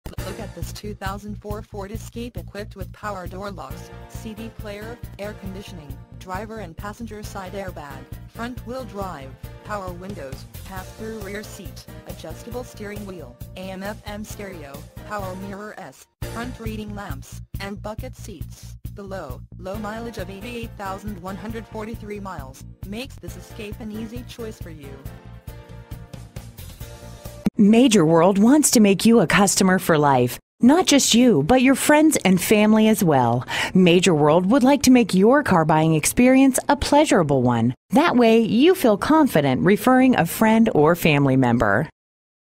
used Ford Escape NY New York 2004 located in Long Island City at Major World
This used Ford Escape NY New York 2004 is located in Long Island City and is offered by Major World and can be compared to any used Ford Escape :